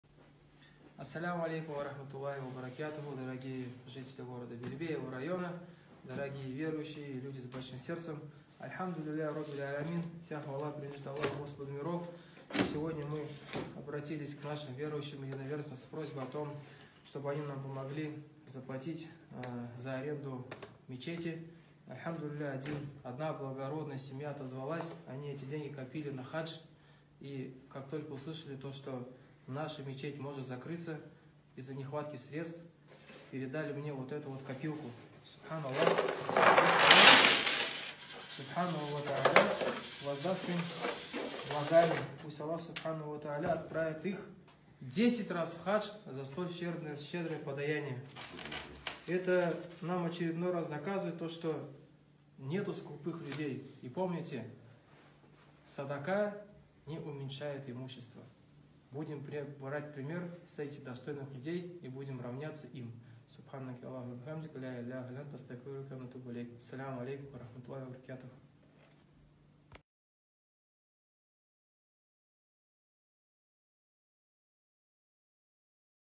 Благое дело
Ассаламу алейкум Ва РахматуЛлахи ва баракятух дорогие жители города Белебее и его района альхамдулиллях Роббиль алямин нам сегодня было пожертвованы денежные средства в размере 20 000 руб. которые мы инша Алла потратим на арендную плату за мечеть.         Хочется отметить не скупость этой семьи которая не пожалела пожертвовать на пути Аллаха такую крупную сумму  денег которую они копили на поездку в Хадж( поломничесво в Мекку)
    Просим Аллаха чтобы Он Субханаху ва Та’аля ( Пречист Он и возвышен )даровал им средства на несколько поездок в Хадж .
  Также призываю вас дорогие верующие ,дорогие наши друзья ,дорогие люди с большим сердцем ,чтобы вы усердствовали на пути Аллаха ,чтобы вы усердствовали на хороших делах, чтобы вы не жалели ни сил ни средств на пути всевышнего Аллаха ,дабы изменить этот мир ведь религия ислам пришла для того чтобы в мире царил мир и был порядок .